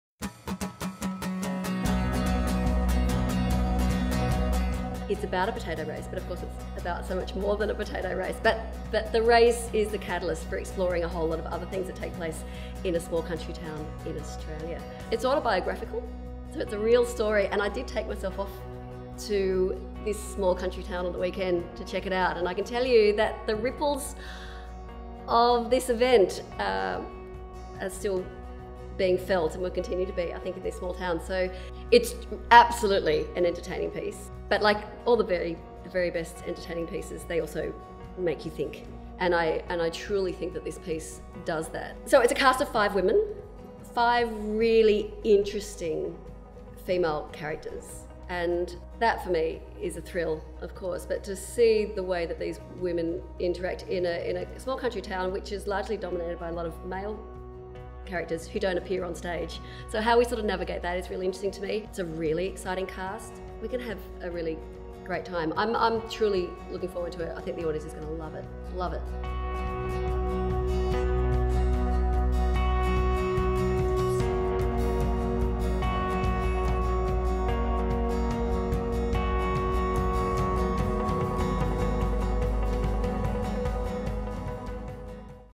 The Appleton Ladies' Potato Race by Melanie Tait
This new Australian comedy, featuring a cast of five fabulous women, is a funny, honest and heart-warming story about upsetting the potato cart and standing up for your principles.